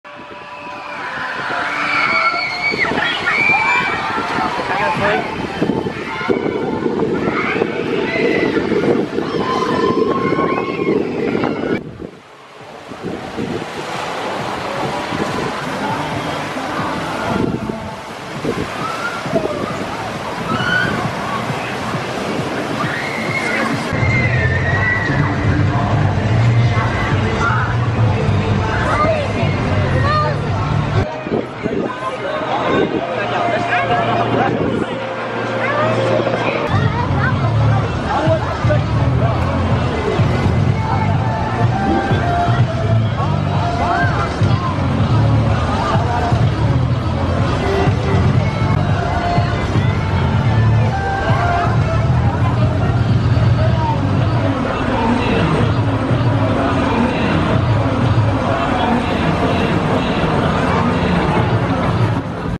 Blue Mountain Mayhem Off-Ride Footage, Soaky Mountain Waterpark TornadoWAVE | Non-Copyright
Blue Mountain Mayhem is a Tornado Wave water slide at Soaky Mountain Waterpark.  This slide features two wall elements as it works its way down the hill.